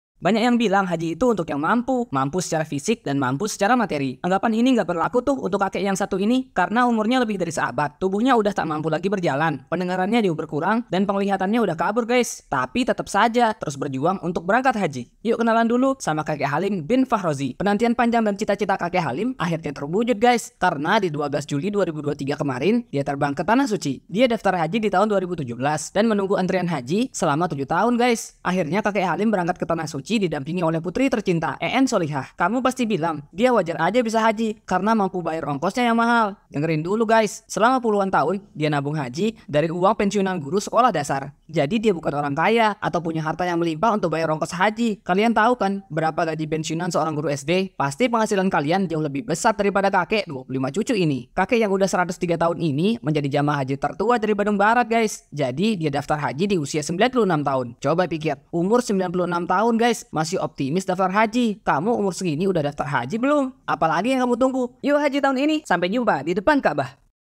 Pensiunan guru SD Naik haji, kisah inspiratif haji 011
Pensiunan guru SD Naik haji akan buat kamu saddar bahwa haji itu adalah kewajiban bagi semua muslim, baik yang muda atau yang tua, yang kaya atau yang miskin. Tidak ada alasan untuk tidak memberikan usaha terbaik untuk haji. Saksikan video Pensiunan guru SD Naik haji ini, semoga kamu bisa naik haji tahun ini yah...